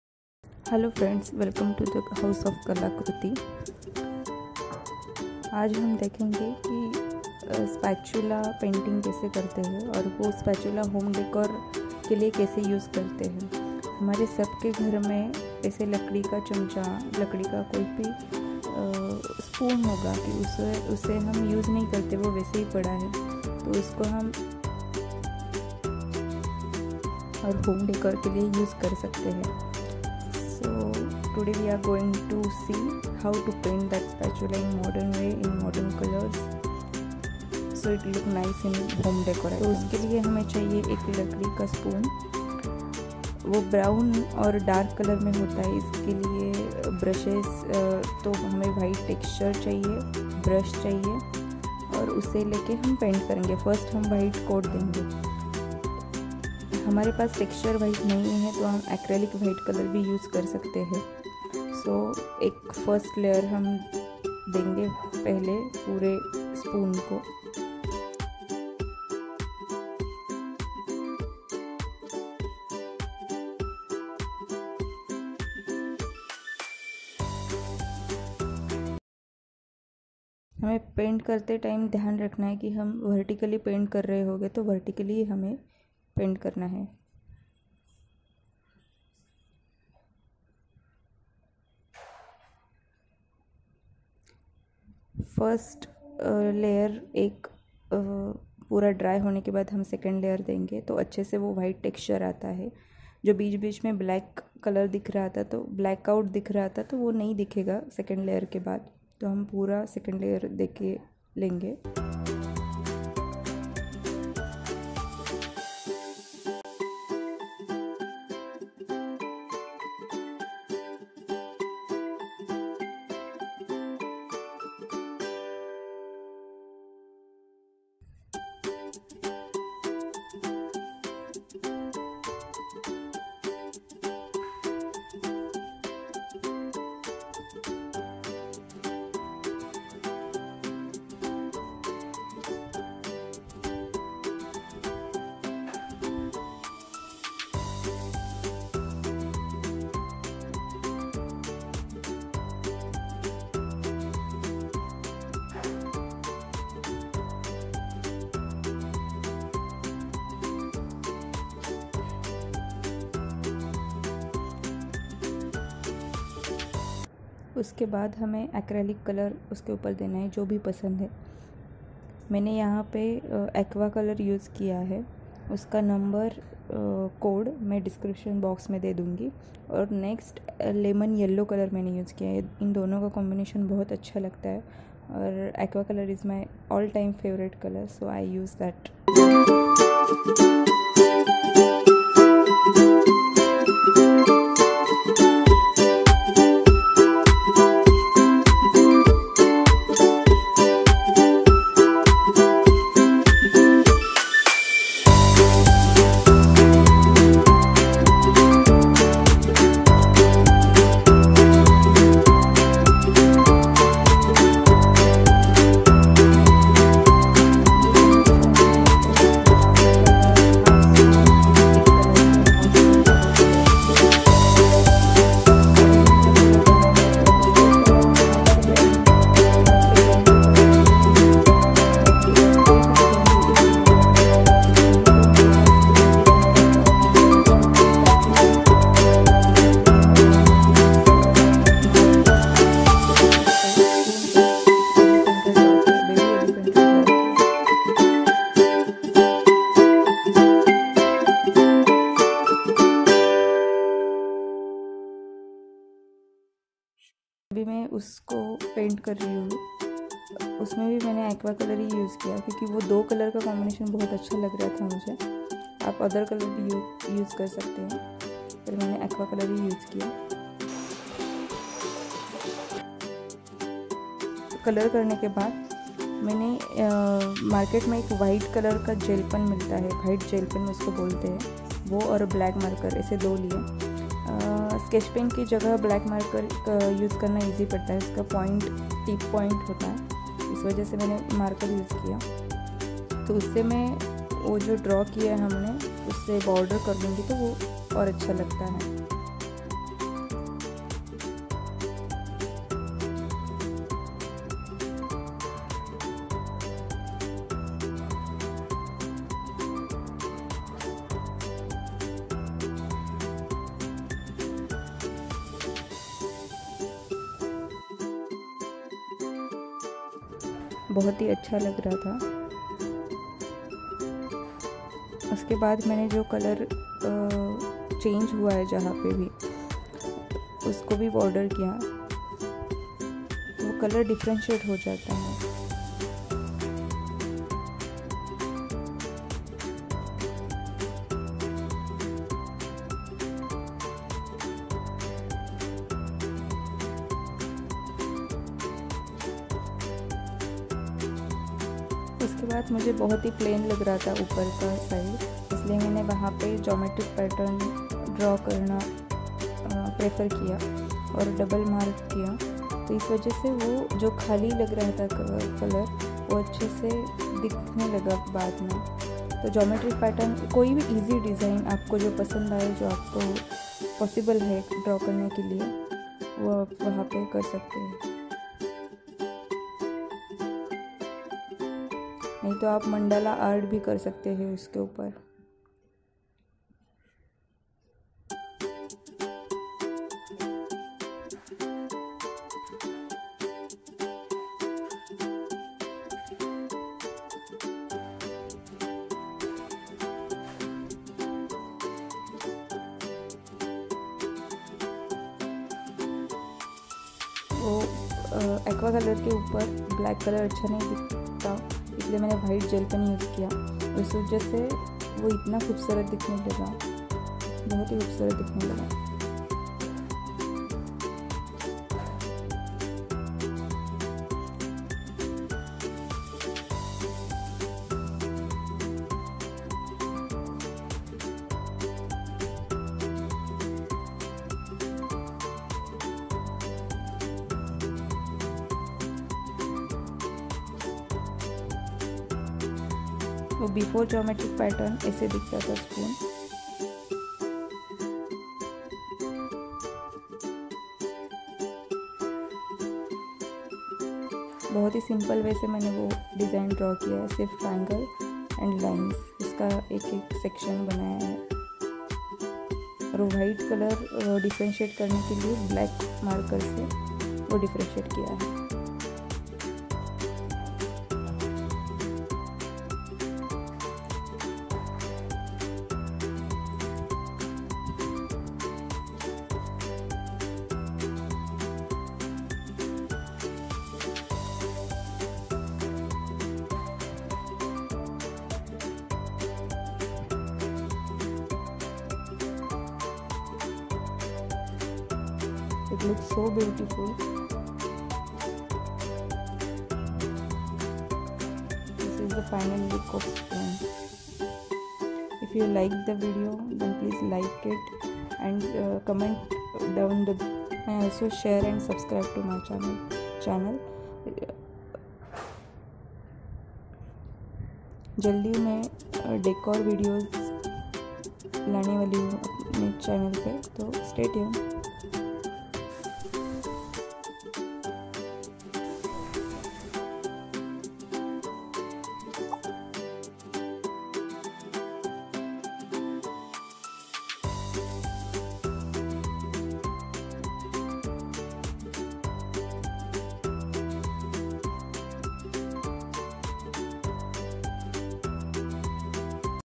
DIY-Wooden Spoon Painting | Wall Hanging Decor ideas |How to Paint on Wooden Spatula
DIWALI DIY-Wooden Spoon Diya stand | Wall Hanging Decor ideas |wooden spoon Painting | Spatulla spoon Painting

Hello friends,
Today i am sharing the video of easy DIY for Diwali decor.I have this spatula in my home and i am not using long time so i decide to paint in modern way.
Every one used for wall hanging ,key holder but i want something different makeover of spatula. Interesting thing is i really know output is what? but within process of painting i get the idea n beautiful transformation of spatula happen.
so lets do it...BE CREATIVE.

Aqua green color code - 009
Lemon Yellow -236

follow me for more decor ideas